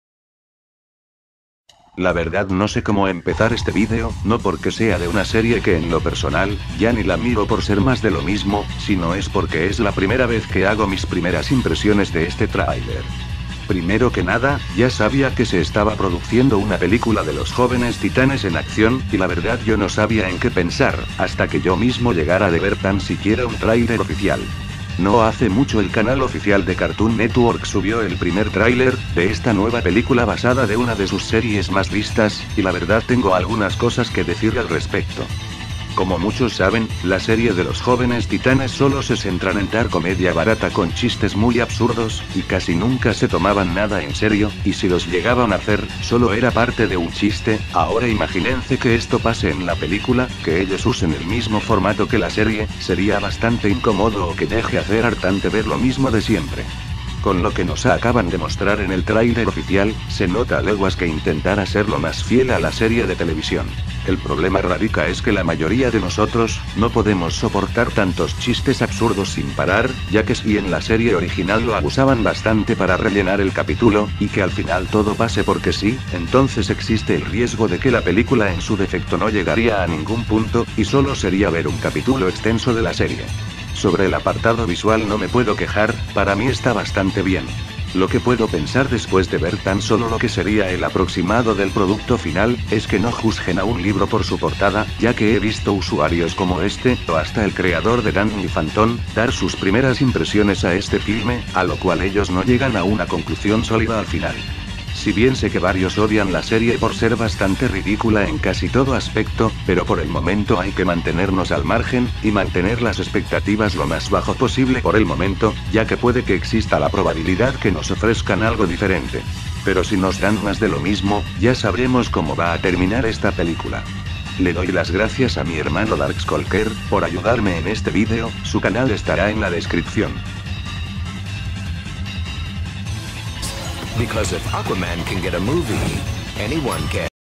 Primeras Impresiones# 2 Jovenes Titanes En Accion La Pelicula (Sera Buena?)
Inicio de la Quinta Temporada del canal
5X01 (1/26)

Primera Vez que hago esto